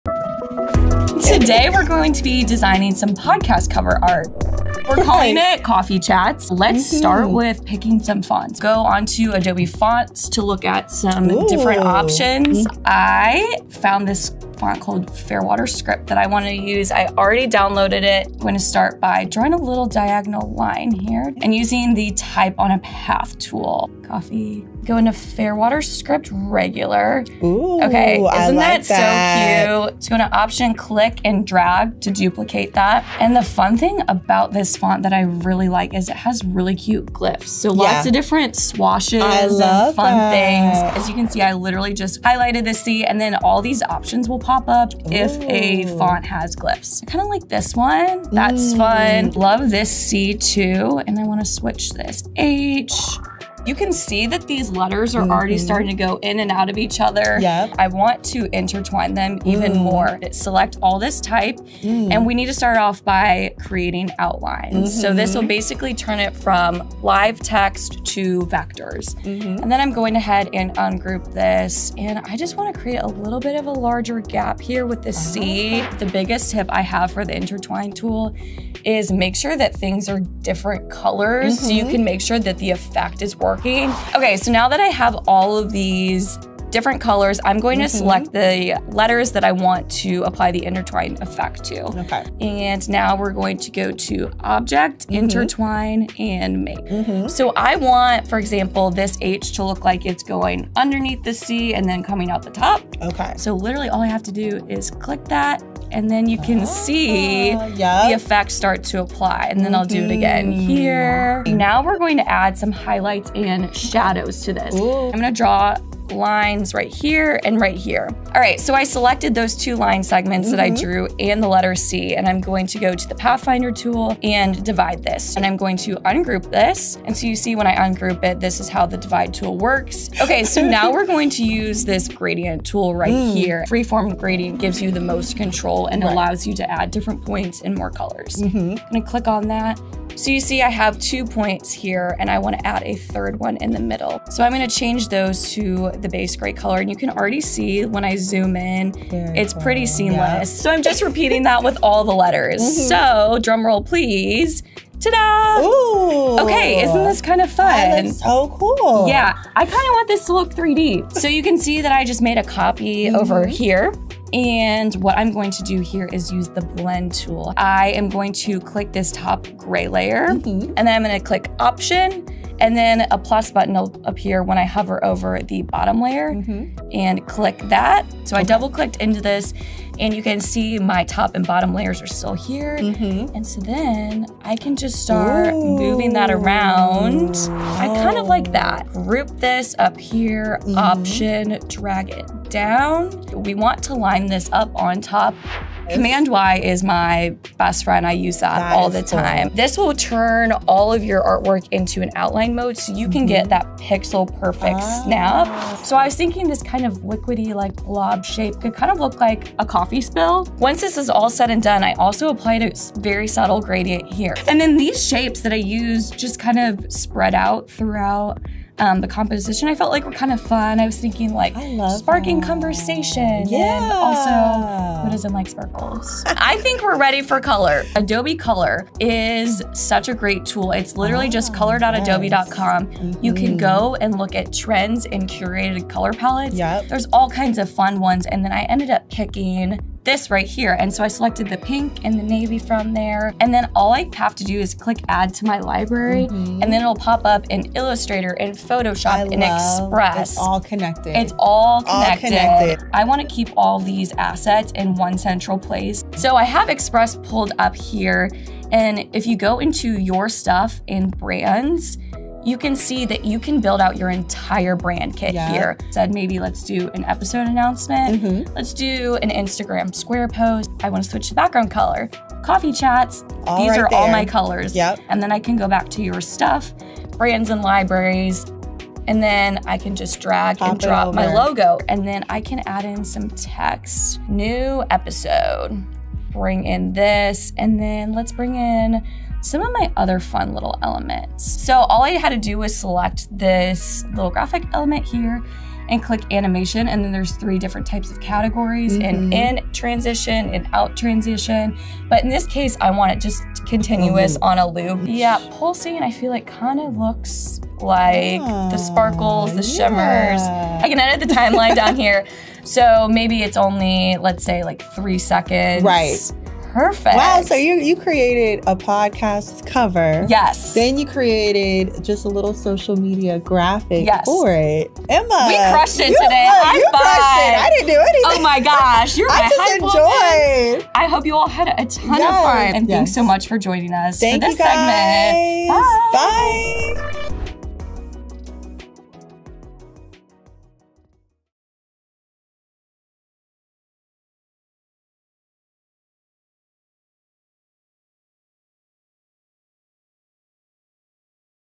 How to Design Podcast Cover Art
Tune in and create custom podcast artwork using Adobe Illustrator. Set up unique artboard sizes, then export directly into Adobe Express to add animation and bring your art to life.